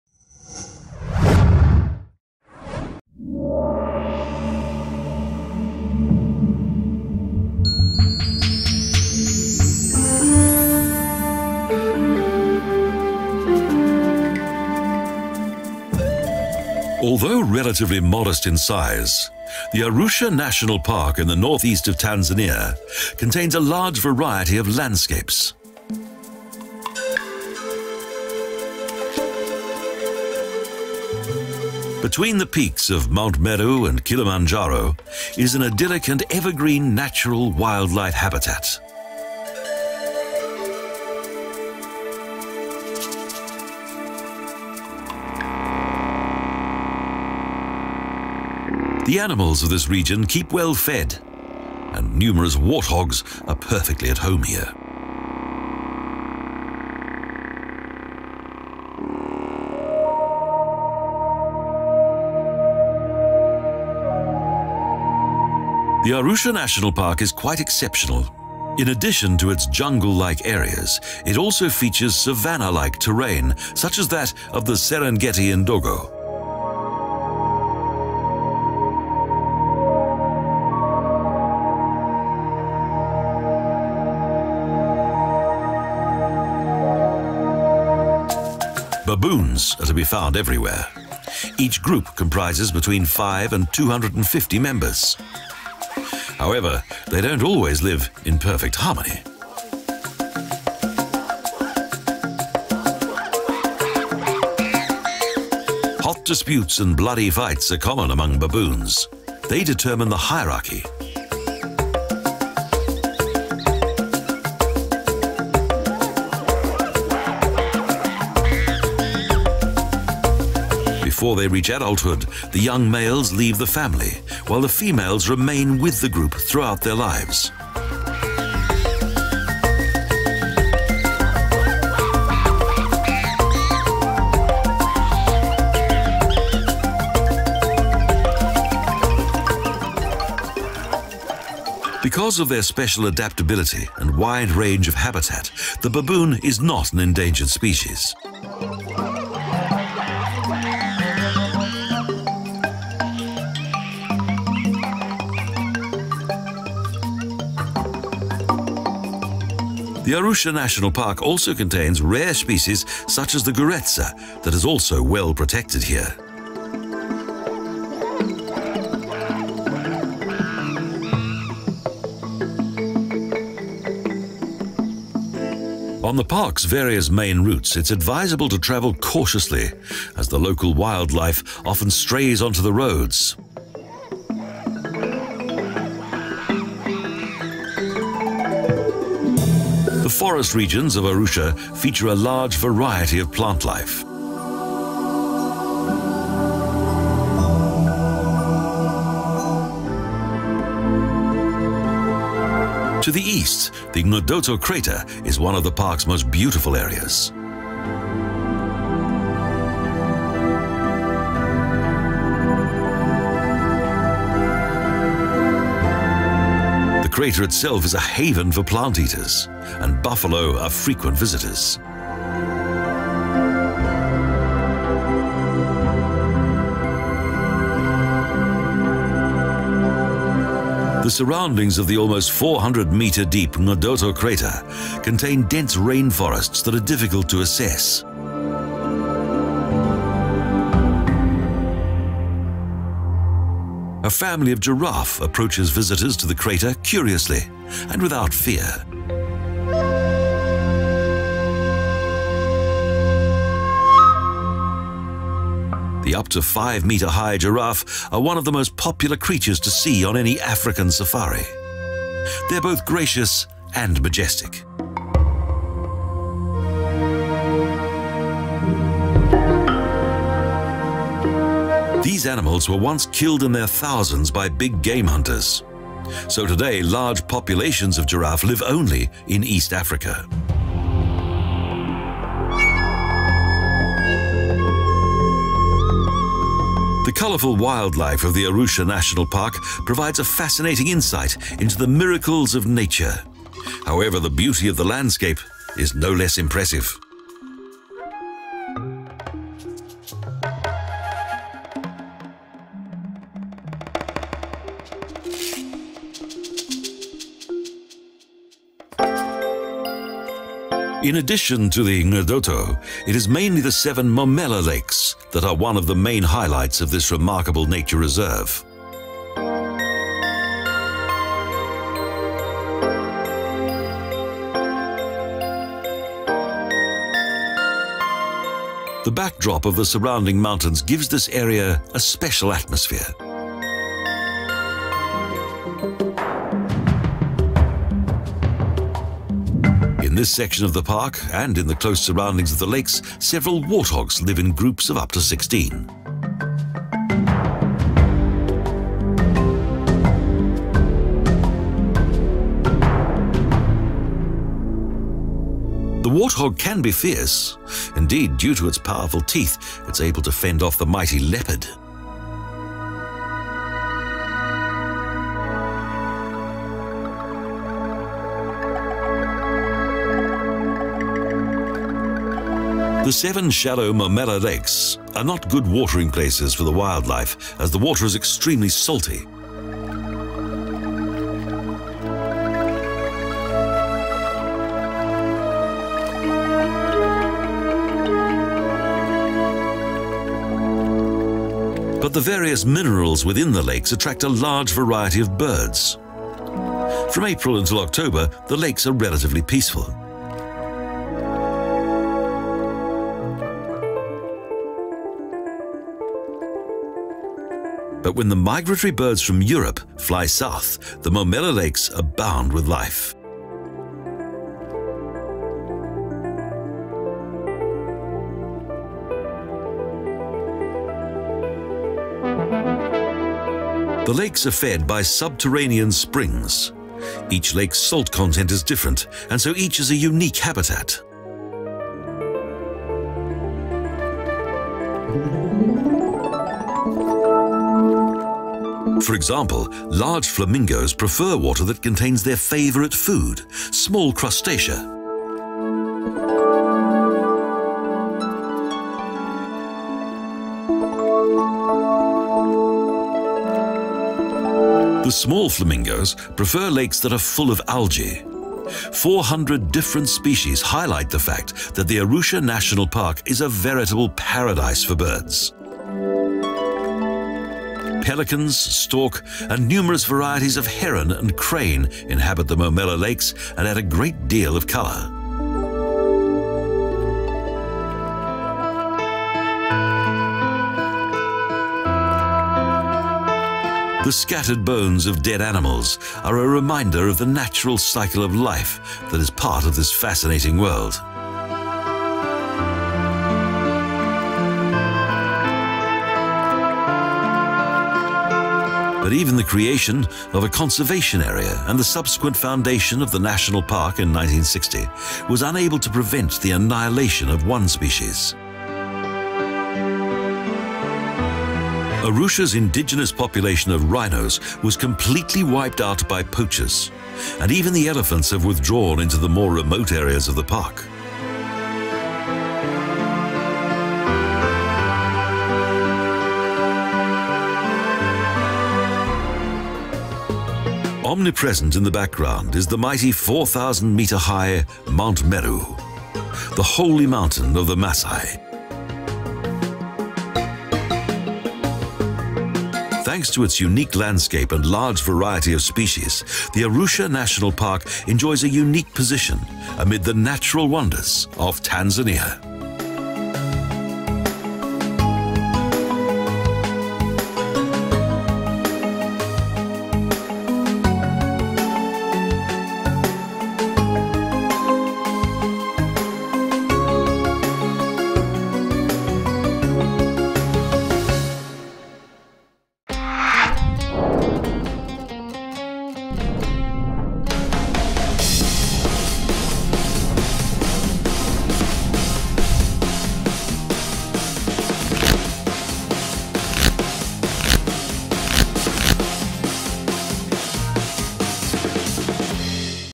Arusha Park Vacation Travel Video Guide
Travel video about destination Arusha Park.
Although relatively modest in size, the Arusha National Park in the north east of Tanzania contains a large variety of landscapes and between the peaks of Mount Meru and Kilimanjaro is an idyllic and evergreen natural wildlife habitat.